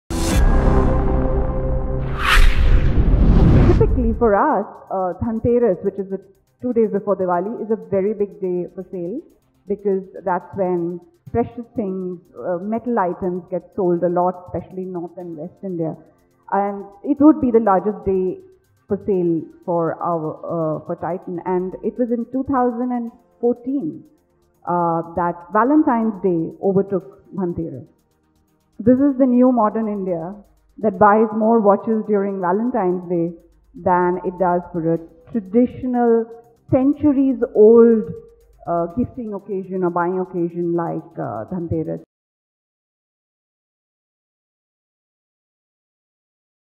Ms. Suparna Mitra, Titan Company Ltd at the QCGC17
"In 2014, Valentine's Day overtook Dhanteras in gifting" says Ms.Suparna Mitra, Chief Marketing Officer, Titan Company Ltd, commenting on a remarkable shift in the behaviour of the Indian consumer. Listen to more such unique insights on the Indian gifting industry at the Qwikcilver Gift Card Conclave.